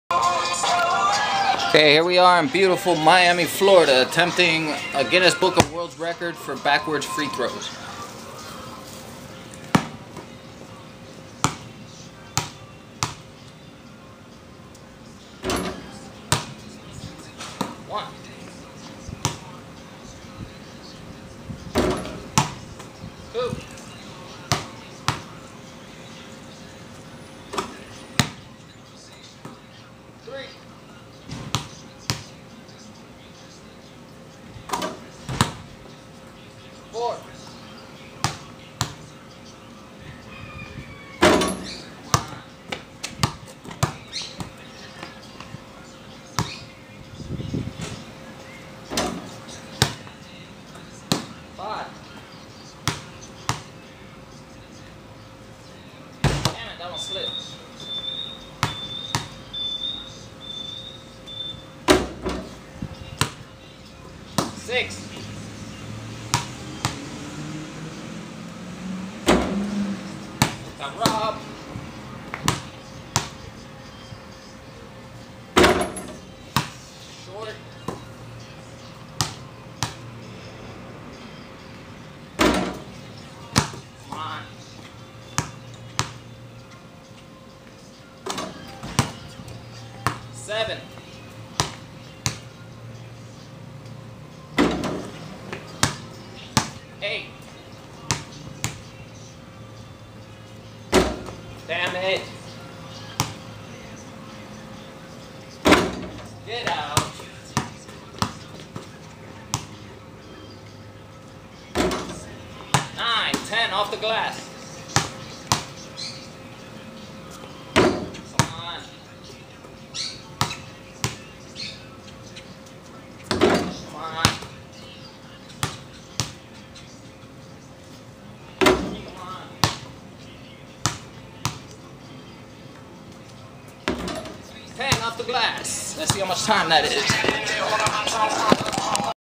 10 NBA Free Throws Backwards.MP4
Guiness Book of World Records come holla at me and let's make this official lol
On my first day of trying I made 10 NBA Free Throws Backwards in less than 2 minutes. This is an unedited video, and I did this with no help from anyone. Imagine what I could do with some help.
Guiness
World
Record
NBA
Free
Throw
Backward